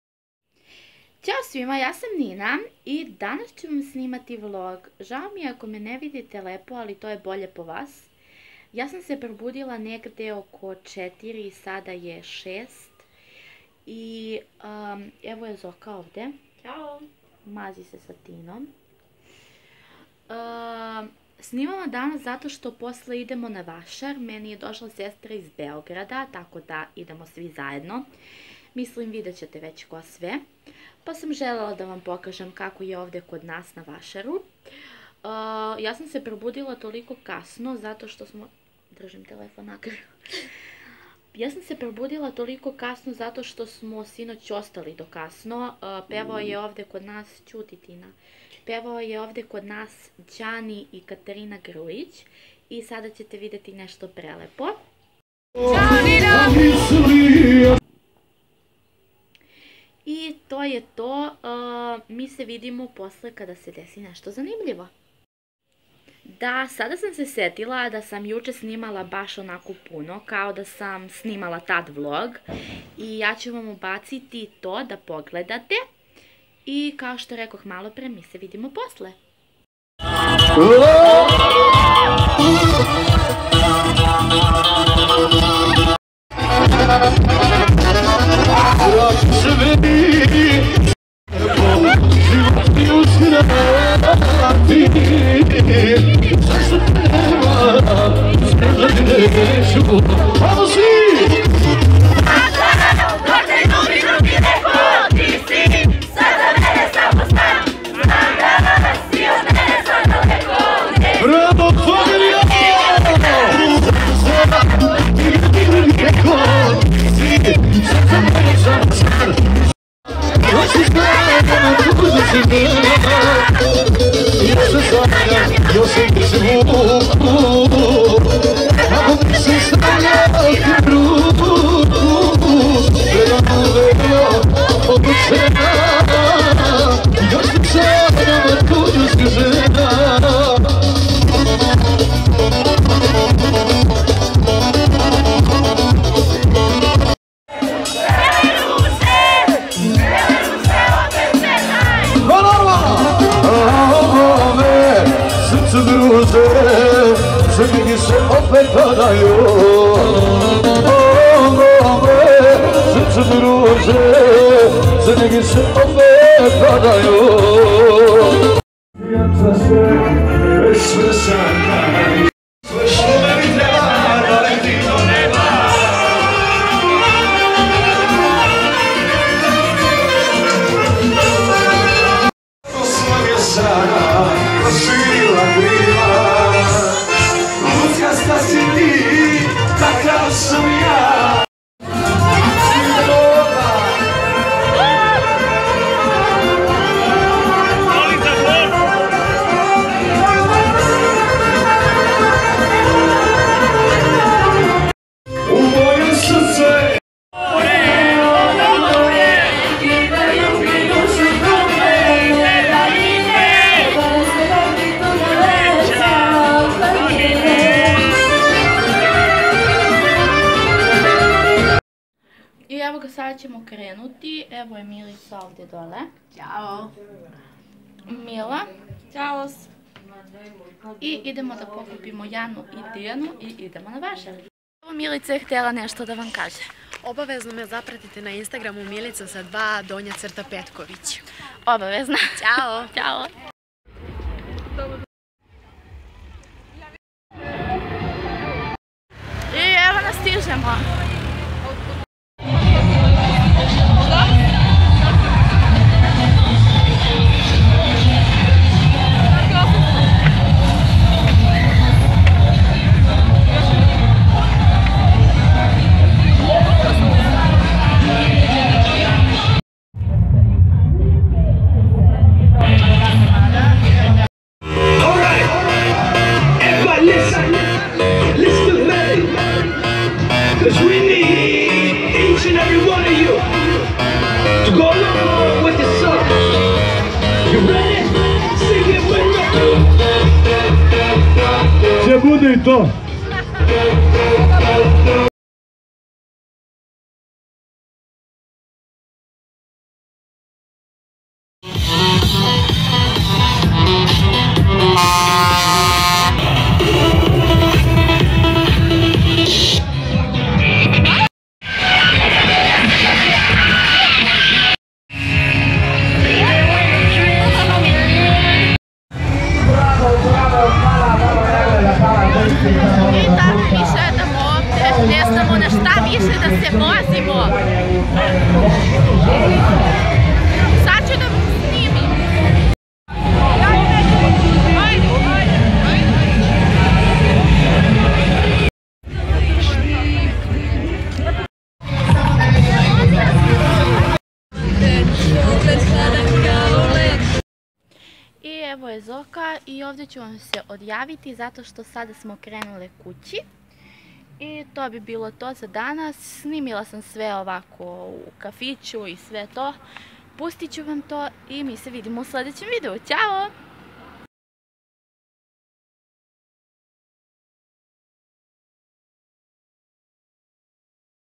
VASAR | KATARINA GRUJIC U VLOGU
Zao mi je ako niste uspeli sve da vidite lepo ,ali sam zbog guzve sve snimila brzo , nadam se da mi necete zameriti :)
Vidimo se u sledecem videu , ljubim vas sve